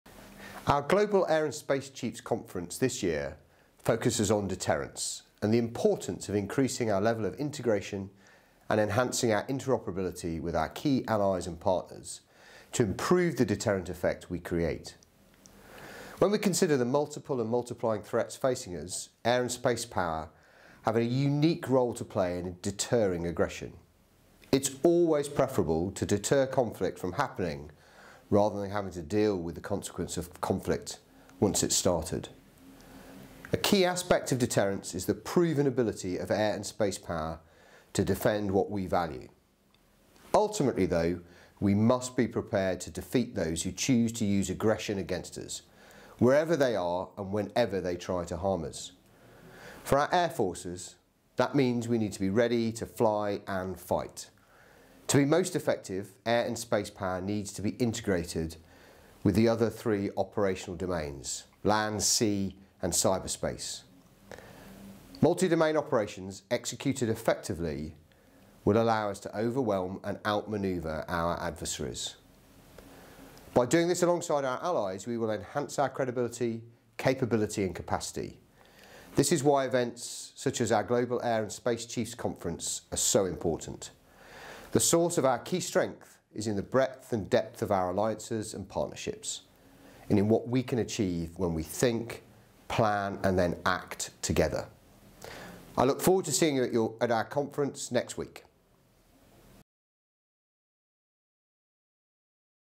ACM Sir Richard Knighton, Chief of the Air Staff on the Global Air & Space Chiefs’ Conference
Air Chief Marshal Sir Richard Knighton, Chief of the Air Staff on the Global Air & Space Chiefs’ Conference